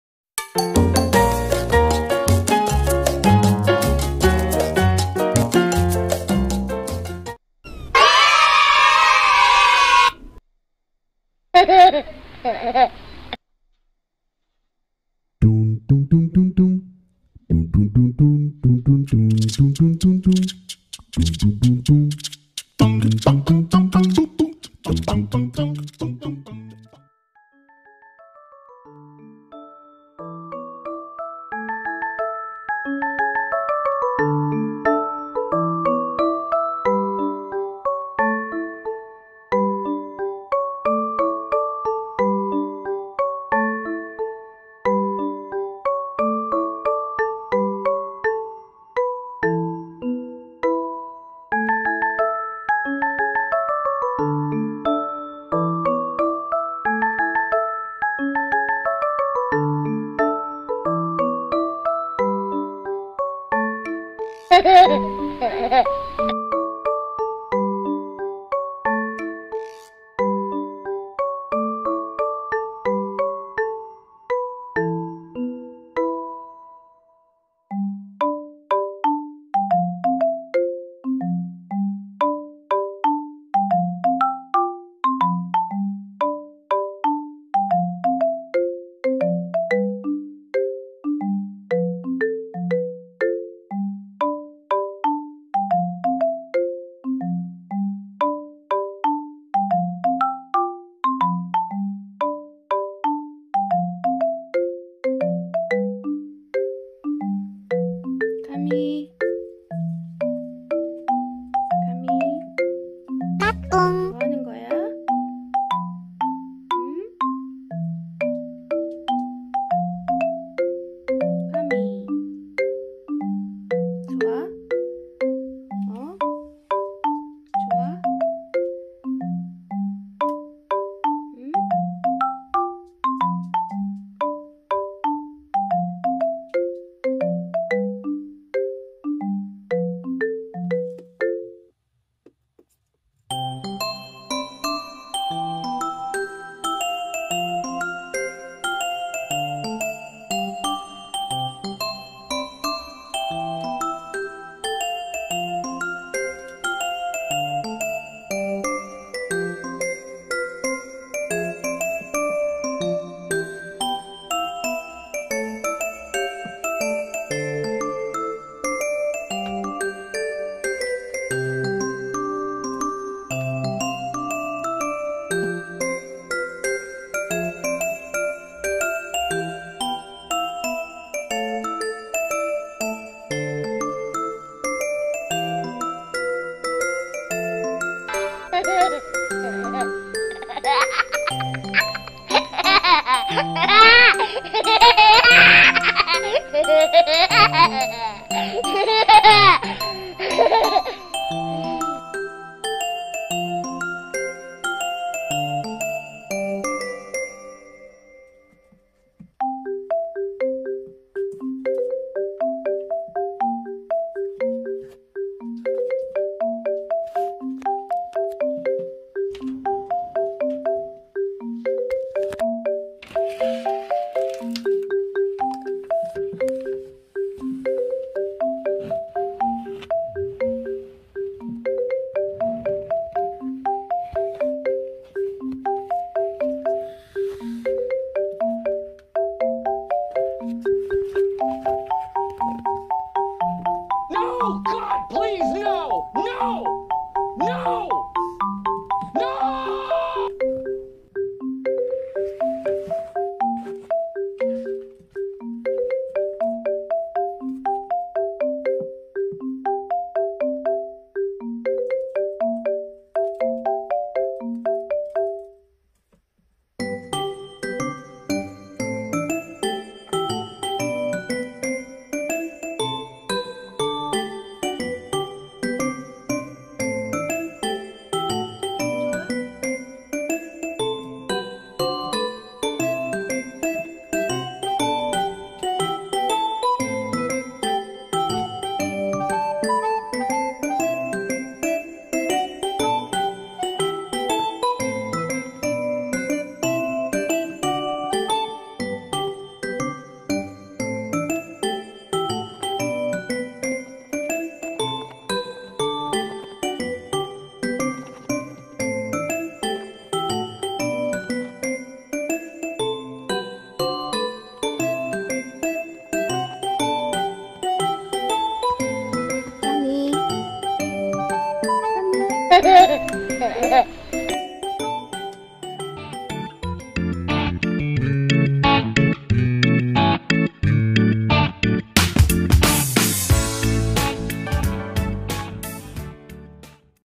프레리독이빨_의힘 이구역에_미친애는_바로_나 파티하는_프레리독
감이가 자꾸 벽을 타고 떨어져서 다리를 다친적이 많았어요
그래서 바디필로우를 바닥에 둘러 깔아 주었더니...!